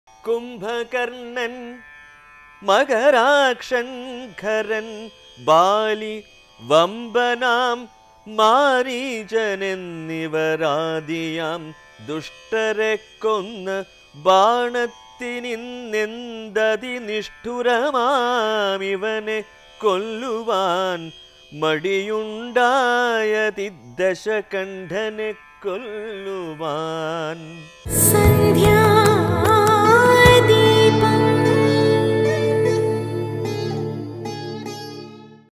ത്യാഗമെന്നാൽ ഉപേക്ഷിക്കൽ അല്ല അതിജീവിക്കൽ ആണ് .
ത്യാഗമെന്നാൽ ഉപേക്ഷിക്കൽ അല്ല അതിജീവിക്കൽ ആണ് .ശ്രീ കാവാലം ശ്രീകുമാറിന്റെ രാമായണ പാരായണം കേൾക്കാം .